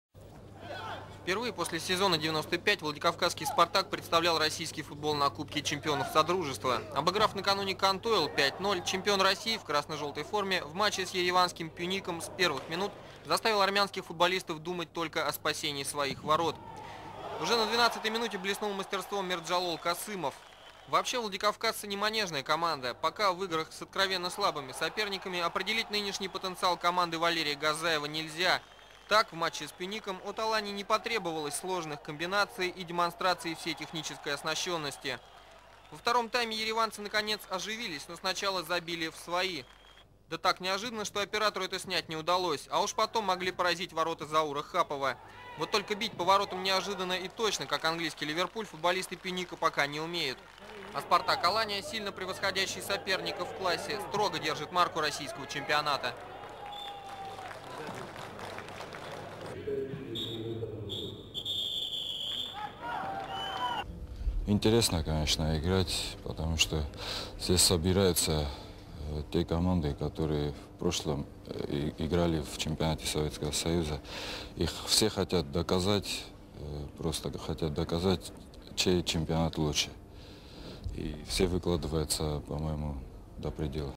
[27.01.1996] Alanya Spartak - Pyunik Yerevan
[Kasymov 11, Suleymanov 12, Yanovsky 15, Gevorkyan 50 own goal]
attendance: 2200
Yellow card: Korniyenko